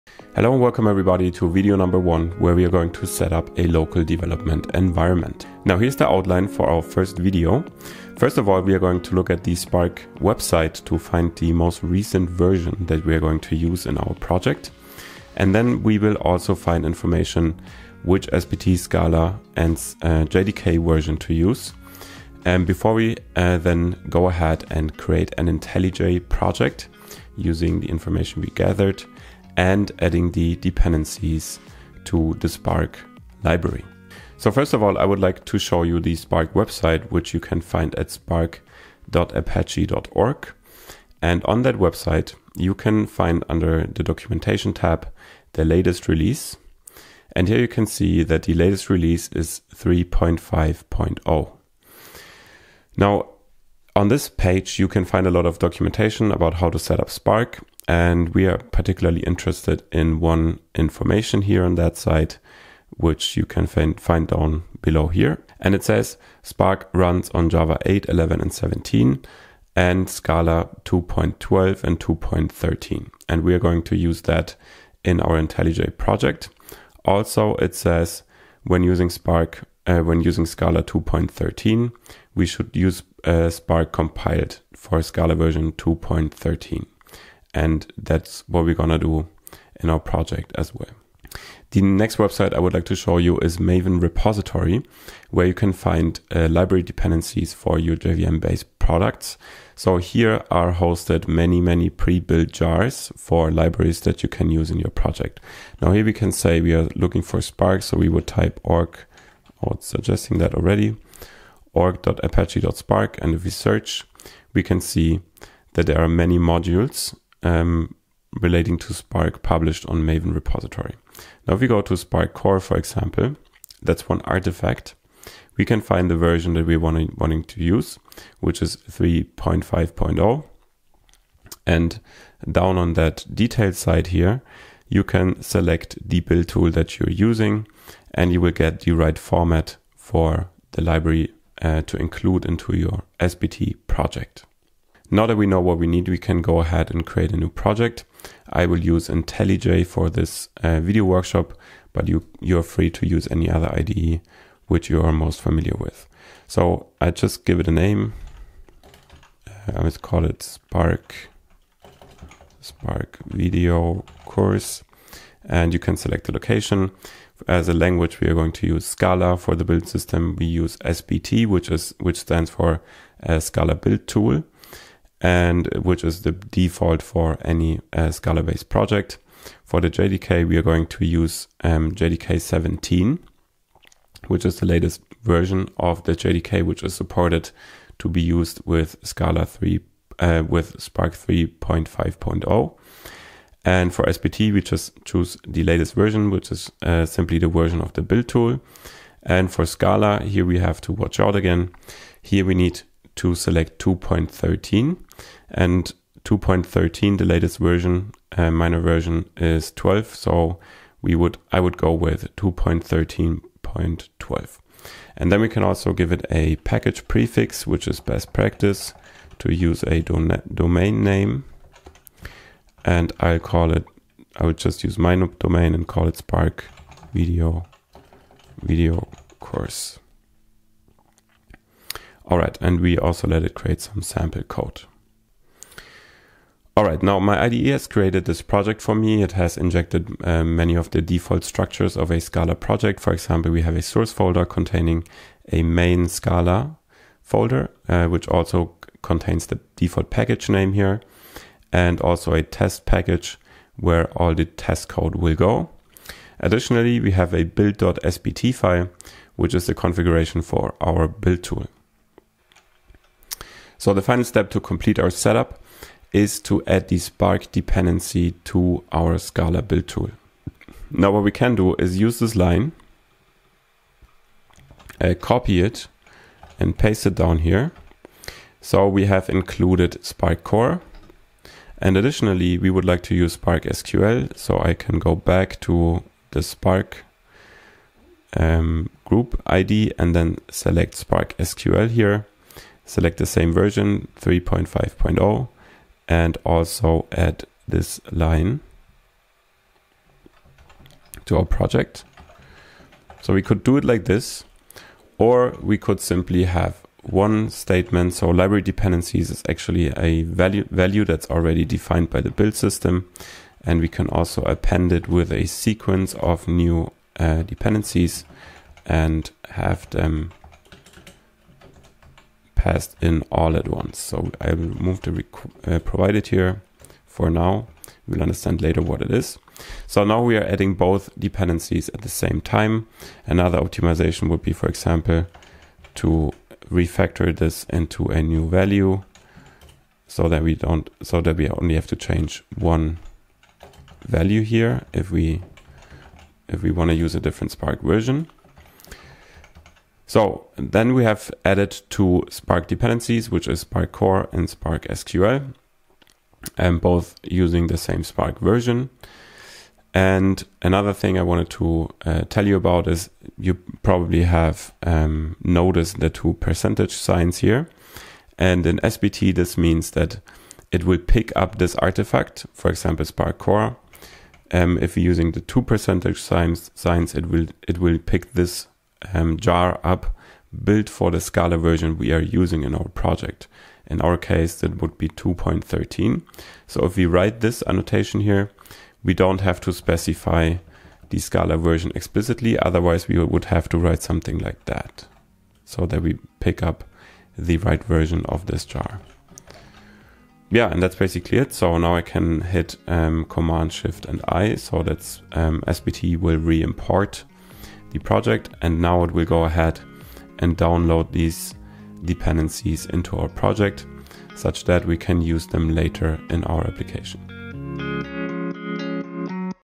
Spark with Scala Course - #1 Setting up a development environment using IntelliJ, SBT, Scala & Spark
Apache Spark with Scala Course: From Beginner to Pro
Video 1: Setting up a development environment using IntelliJ, SBT, Scala & Spark

We will use this setup throughout the entire course. You will also learn where to find versions to use in the official documentation.

00:00 Intro
00:34 Exploring the official documentation of Spark
01:30 Finding SBT dependencies
02:24 Creating an IntelliJ project
04:22 Adding Spark to the project
06:30 Conclusion